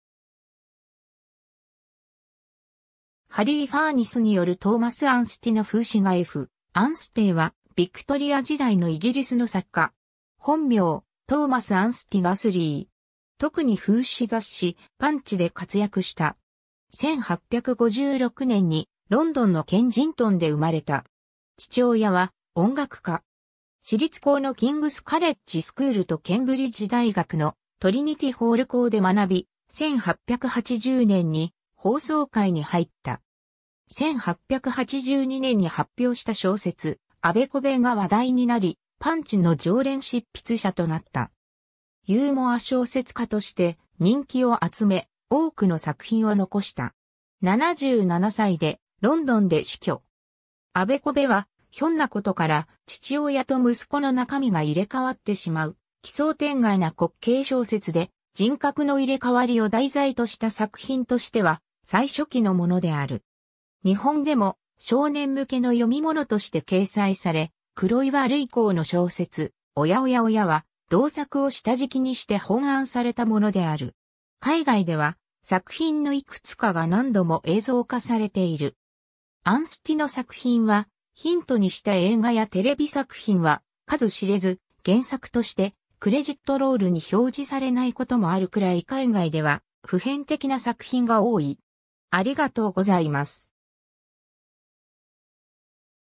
エフ・アンスティ
ハリー・ファーニスによるトーマス・アンスティの風刺画 エフ・アンステイ（F. Anstey, 1856年8月8日 - 1934年3月10日）は、ビクトリア時代のイギリスの作家。
本名、Thomas Anstey Guthrie。
とくに風刺雑誌『パンチ』で活躍した。
 1856年にロンドンのケンジントンで生まれた。
父親は音楽家。
私立校のキングス・カレッジ・スクールとケンブリッジ大学のトリニティ・ホール校で学び、1880年に法曹界に入った。
1882年に発表した小説『あべこべ（Vice Versa）』が話題になり、『パンチ』の常連執筆者となった。
ユーモア小説家として人気を集め、多くの作品を残した。
77歳でロンドンで死去。
 『あべこべ』は、ひょんなことから父親と息子の中身が入れ替わってしまう奇想天外な滑稽小説で、人格の入れ替わりを題材とした作品としては最初期のものである。
日本でも少年向けの読み物として掲載され、黒岩涙香の小説『おやおや親』は同作を下敷きにして翻案されたものである。
 海外では、作品のいくつかが何度も映像化されている。
アンスティの作品は、ヒントにした映画やテレビ作品は数知れず、原作としてクレジットロールに表示されないこともあるくらい海外では普遍的な作品が多い。